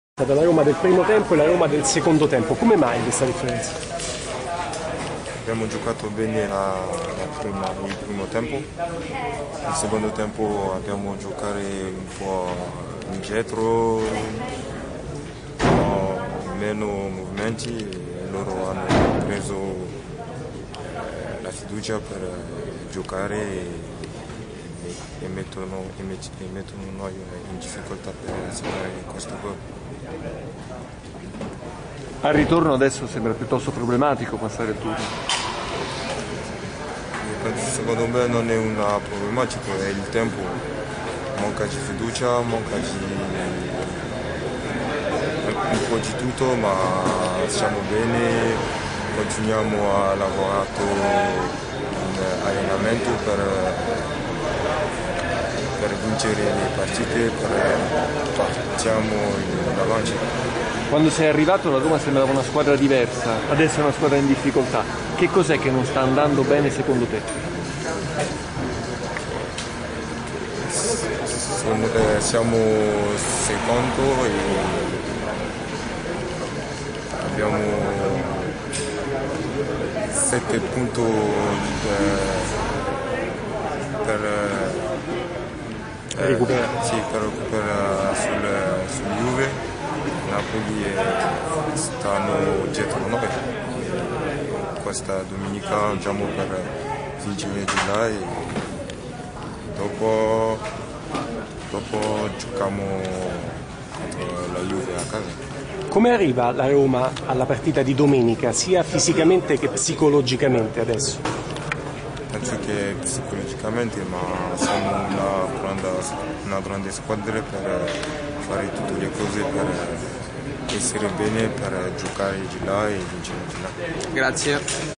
Roma Feyenoord: Mapou Yanga Mbiwa in mixed zone
Le parole del difensore giallorosso al termine dell'andata di Europa League.